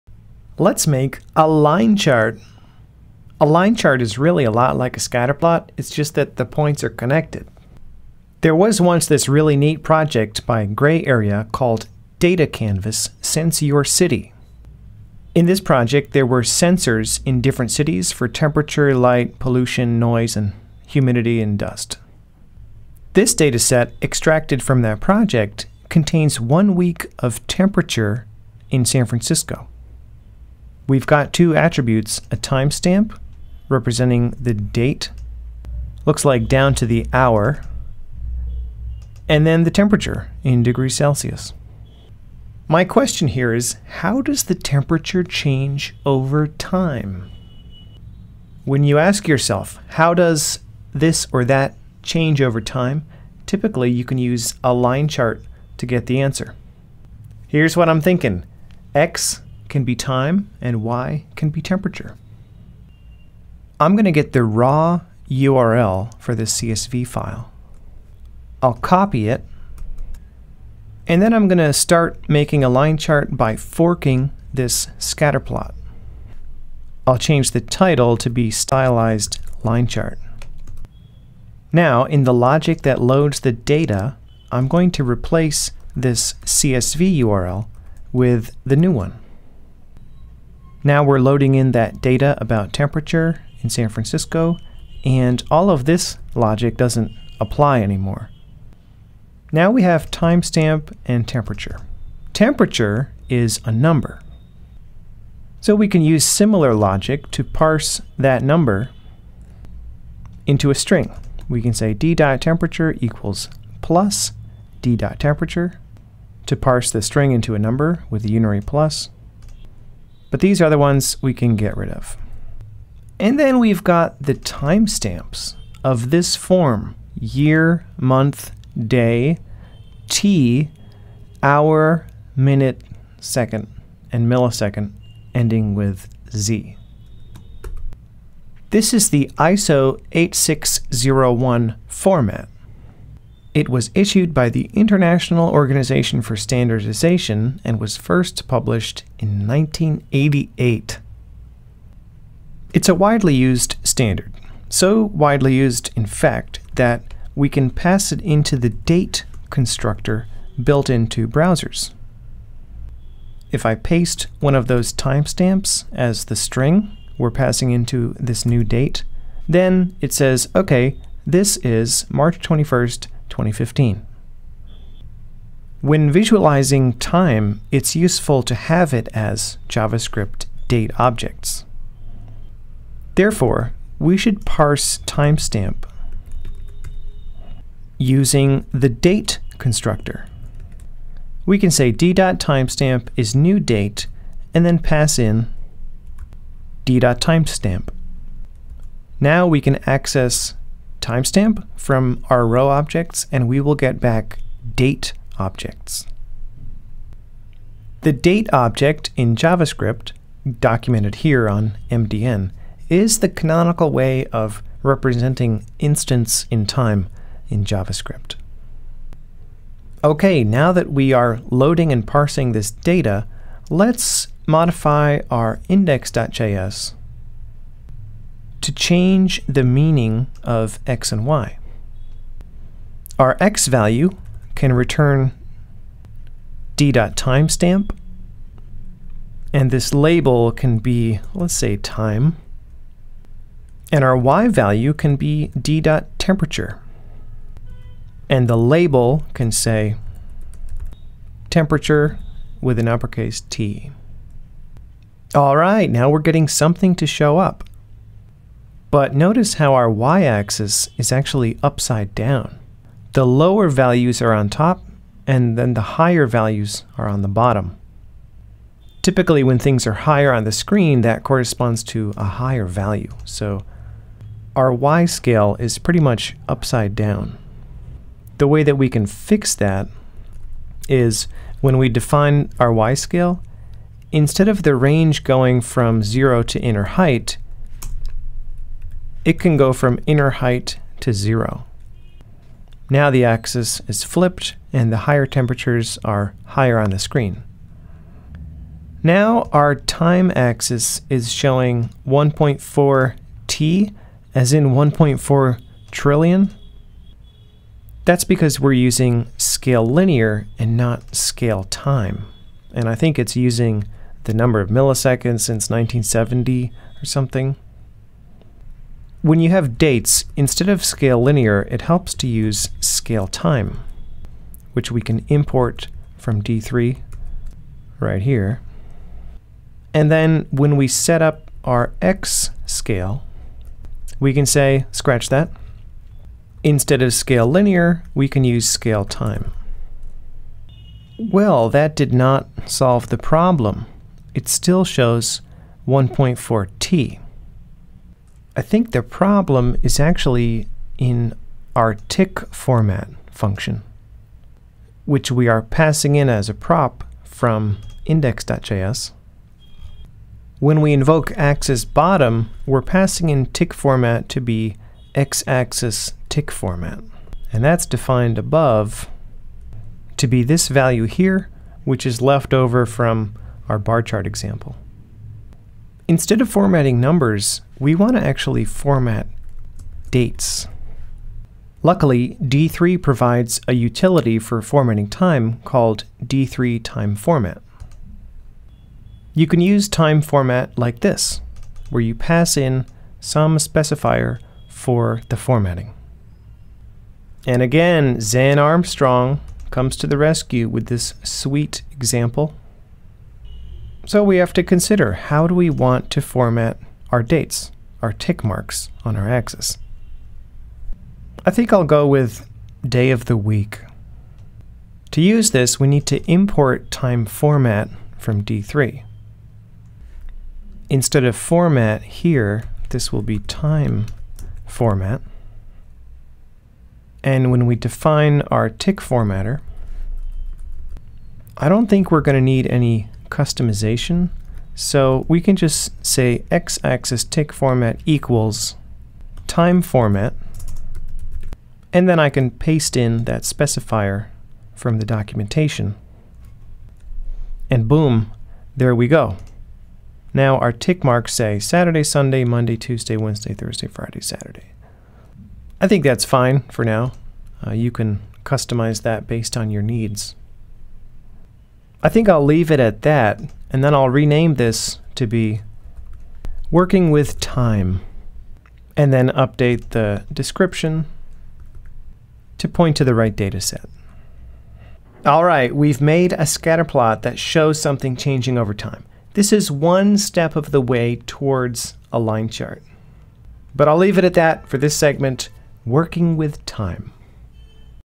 Datavis 2020 Episode 26 Working with Time
Learn how to work with time using D3!

00:00 Intro to dataset
01:15 Fork and load new dataset, refactor
01:39 Parse timestamp, temperature
02:19 ISO 8601 Date and Time format
03:42 Modify text labels
04:25 Fix Y-Axis orientation
05:00 Fix X-Axis tick text
06:15 Format numbers in D3 with d3.timeFormat